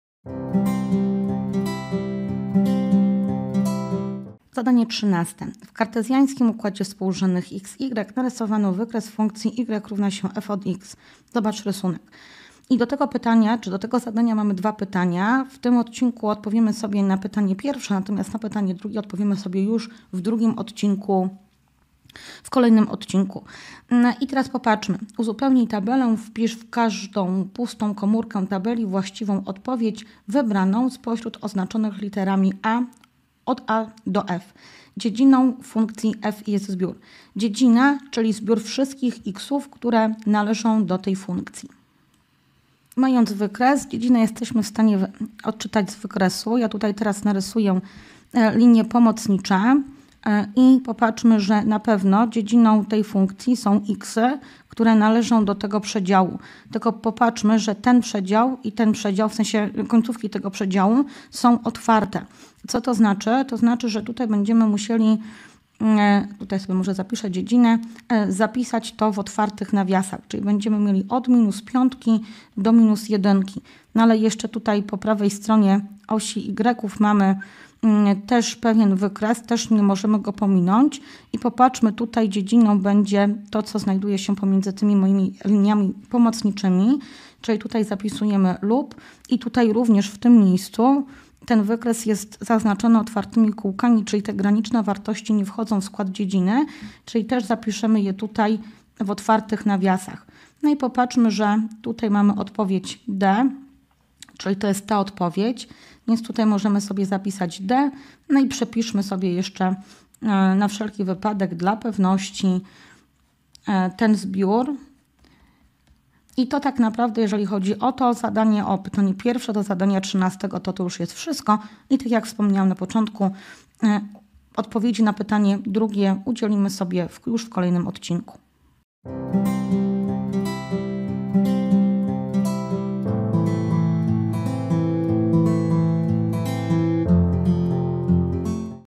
W kartezjańskim układzie współrzędnych (x,y) narysowano wykres funkcji y=f(x) (zobacz rysunek).
Matura dodatkowa - czerwiec 2023. Zadanie 13.1